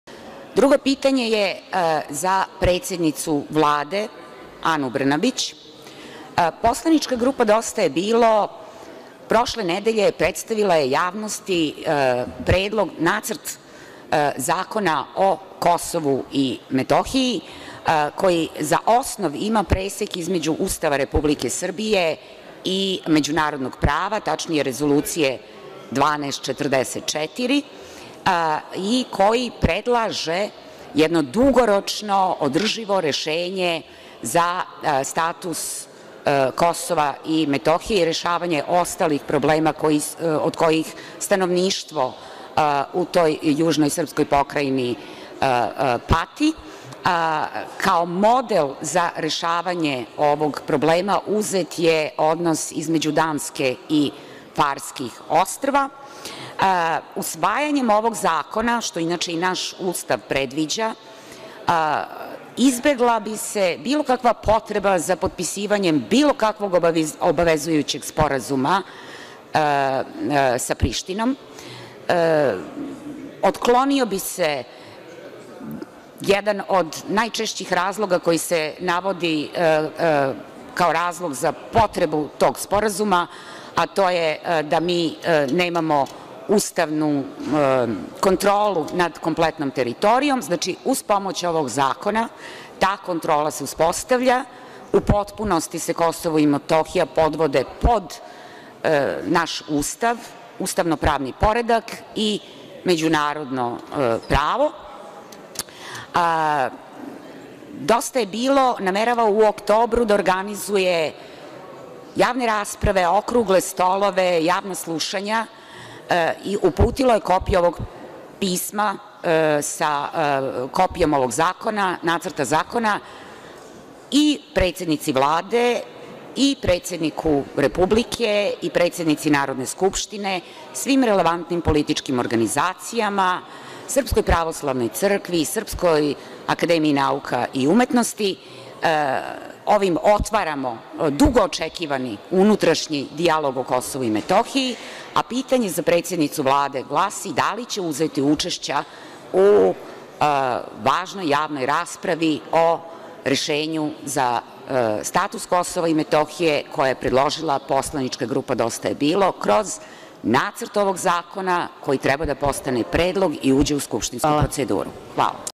Da li će premijerka Brnabić učestvovati na javnoj raspravi o zakonu o KiM?
Poslanička grupa Dosta je bilo predstavila je javnosti Nacrt zakona o Kosovu i Metohiji koji za cilj ima rešavanje pitanja statusa Kosova i Metohije u skladu sa Ustavom Republike Srbije, međunarodnim pravom i Rezolucijom 1244.

DJB je kopiju nacrta ovog zakona uputio Vladi Republike Srbije, predsedniku Srbije, predsednici Narodne skupštine, relevantnim političkim strankama, Srpskoj akademiji nauka i umetnosti, Srpskoj pravoslavnoj crkvi i drugim relevantnim organizacijama i pojedincima u društvu, uz poziv za učešće u unutrašnjem dijalogu o Kosovu i Metohiji da svojim sugestijama doprinesu izradi konačnog predloga zakona, pre nego što ga poslanička grupa Dosta je bilo uputi u skupštinsku prpoceduru.

Pitanje za premijerku Vlade Anu Brnabić: Da li će učestvovati na javnoj raspravi o nacrtu zakona o KiM koji je napravio DJB?